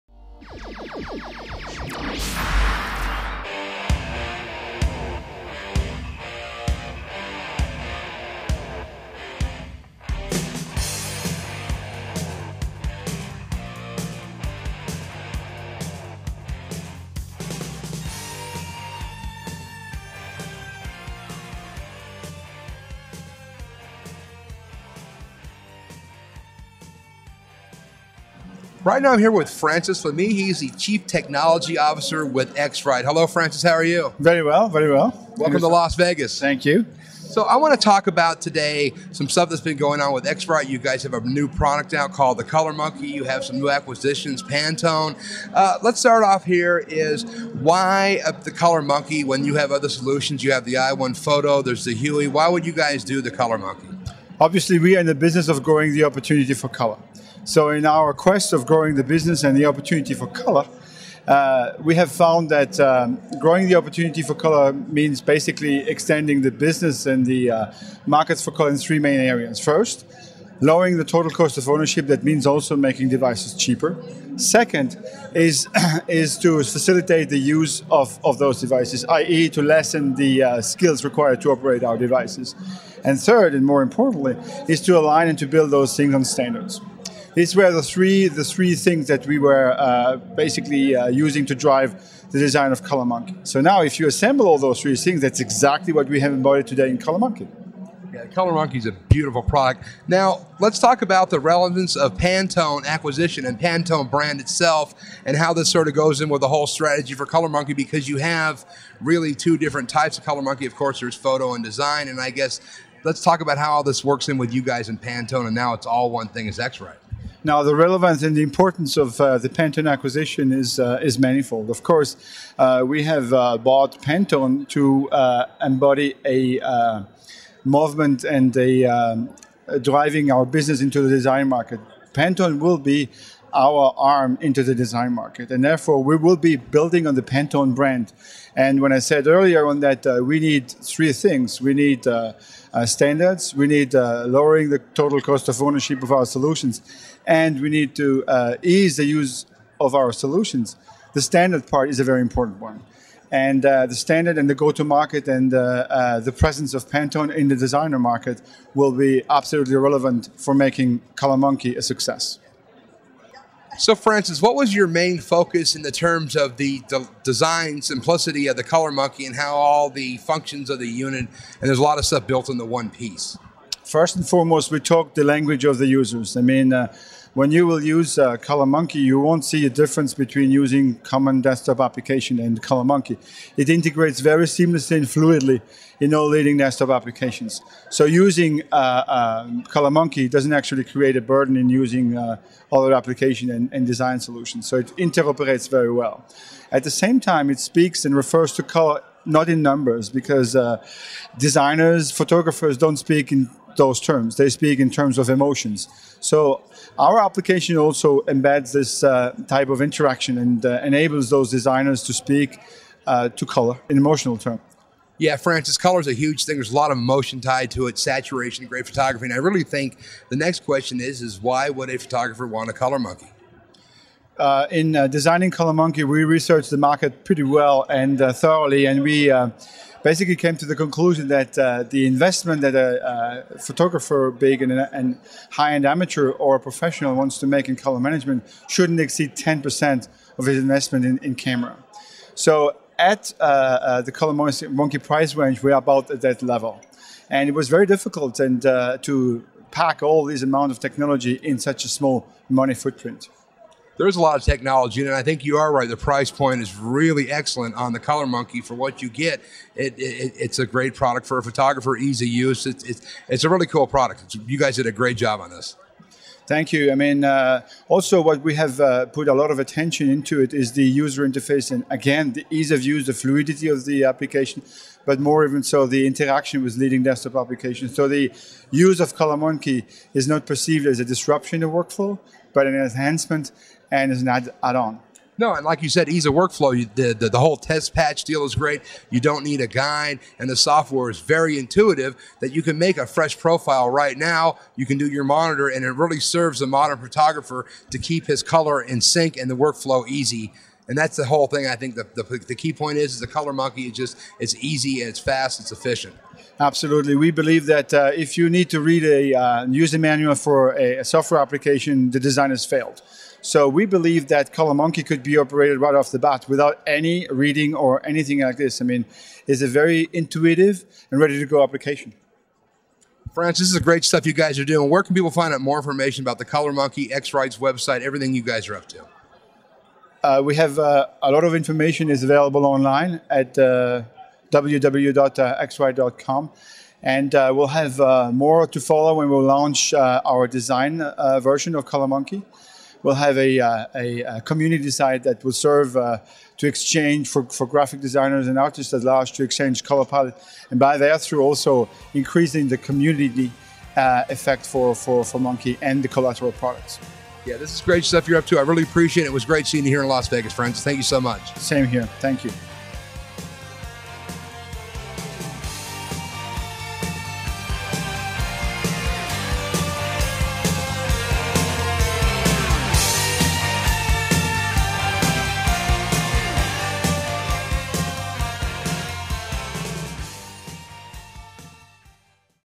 Francis Lamy, X-Rite - IDP TV - WPPI 2008
Francis Lamy, Chief Technology Officer, X-Rite joins Inside Digital Photo TV's Host Scott Sheppard live from their WPPI booth to share their vision and focus discussing the recent acquisition of Pantone and the details around the launch of their new color calibration system, Color Munki Photo. For more information about X-Rite and their full product line